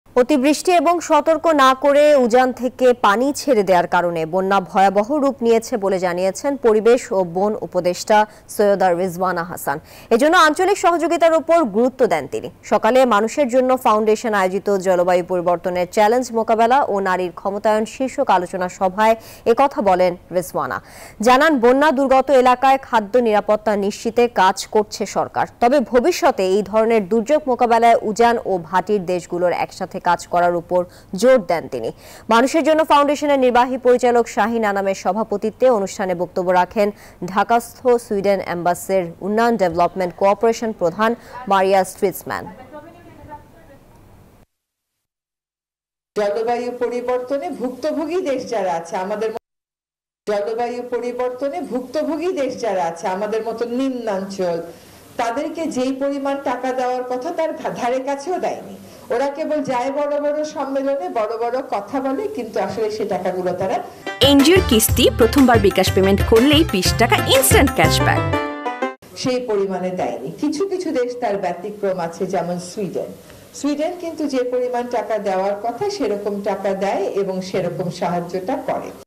জলবায়ু চ্যালেঞ্জ: আঞ্চলিক সহযোগিতা চান পরিবেশ উপদেষ্টা | Climate Change Challenge | News24
NEWS24 Team